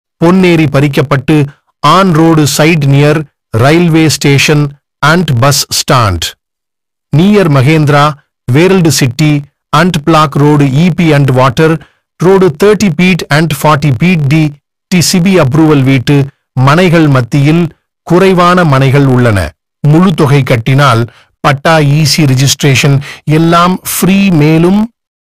வீடுகள் மத்தியில் ரோடு சைடு மனை விற்பனை. பரிக்கபட்டு
வீடுகள் மத்தியில்பொன்னேரி பறிக்கப்பட்டு       ஆன் ரோடு சைட்
நியர் ரயில்வே ஸ்டேஷன்
 அண்ட் பஸ் ஸ்டாண்ட் .
நீயர் மகேந்திரா வேர்ல்டு சிட்டி
டாப் பிளாக் ரோடு
ஈபிஅண்ட் வாட்டர் .
ரோடு 30 பீட் அண்ட் 40  பீட்
டி டி சிபி அப்ரூவல் வீட்டு மனைகள்,
குறைவான மனைகள் உள்ளன ,
முழு தொகை கட்டினால் பட்டா ,
ஈசி, ரிஜிஸ்ட்ரேஷன் எல்லாம் ஃப்ரீ
மேலும் விவரங்களுக்கு
சிங் ப்ராப்பர்ட்டீஸ்